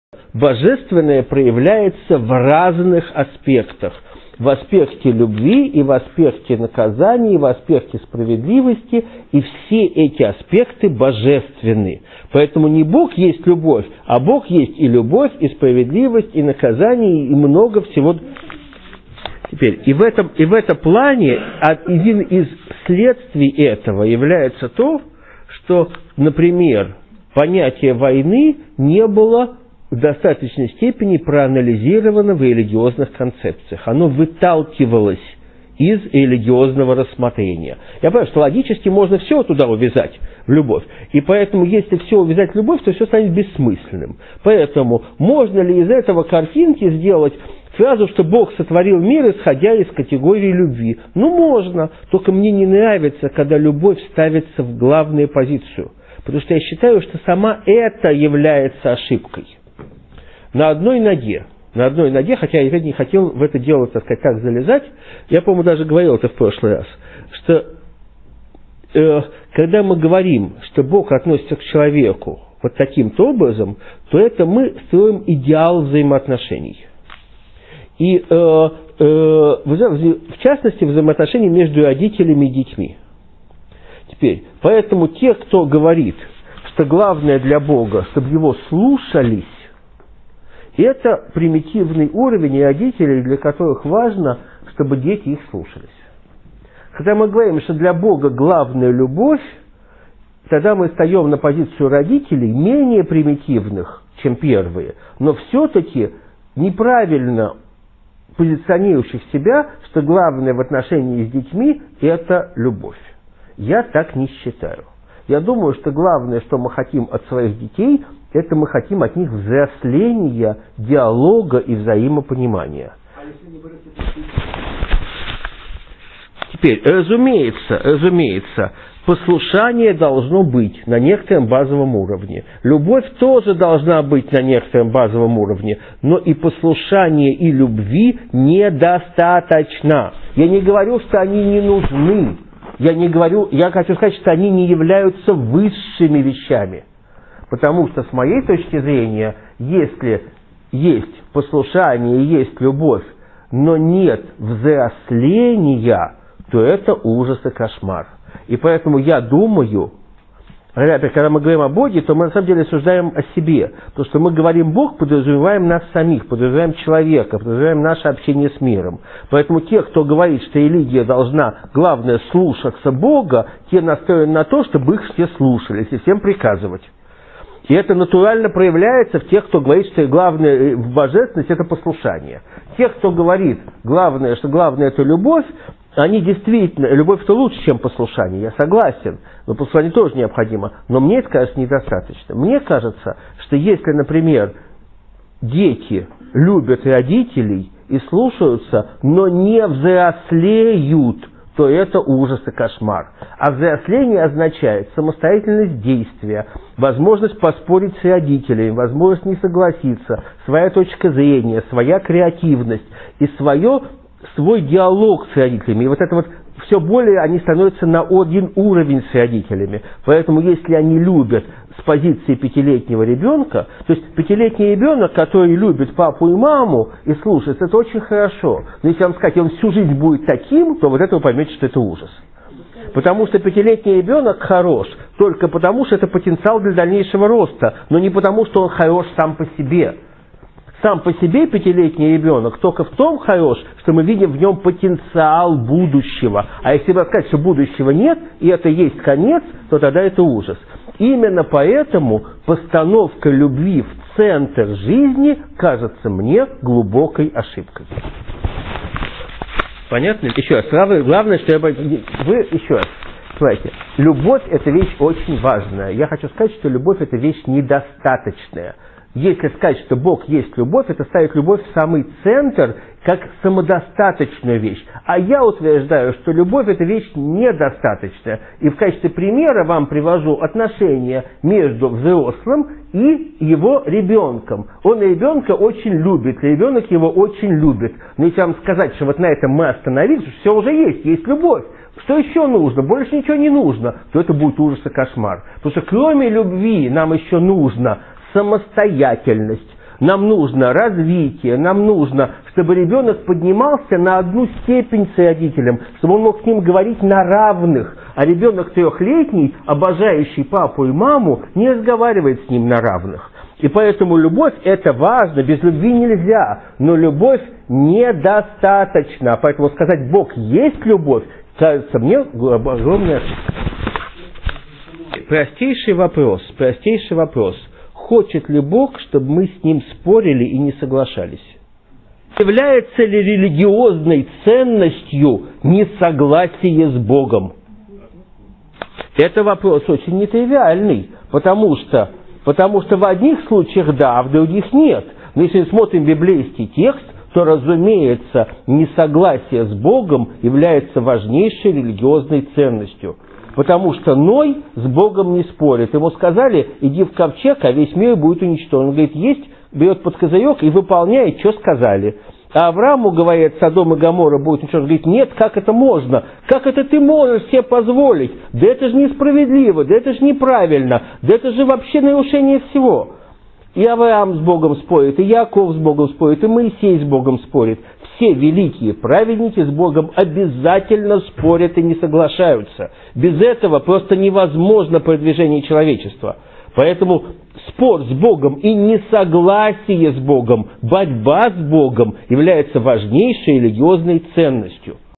Пинхас Полонский: дети должны не только быть послушны и любить родителей, но и спорить с ними
На самом примитивном уровне можно сказать, что важно чтобы бога слушались, точно также как важно чтобы дети слушались родителей. На следующем уровне находится любовь детей к родителям, это уже лучше, но недостаточно. Главное же, что мы хотим от своих детей, это взросления, диалога, и взаимопонимания.

Разумеется послушание должно быть, на некотором базовом уровне. Любовь тоже должна быть на некотором базовом уровне, но послушания и любви недостаточно. Взросление обозначает самостоятельность действия, возможность поспорить с родителями, возможность не согласится, своя точка зрения, своя креативность, свой диалог с родителями, всё более они становятся на один уровень с родителями.

Бог есть и любовь и справедливость и наказание и много всего другого. Спор с богом, несогасие с богом является важнейшей религиозной ценностью.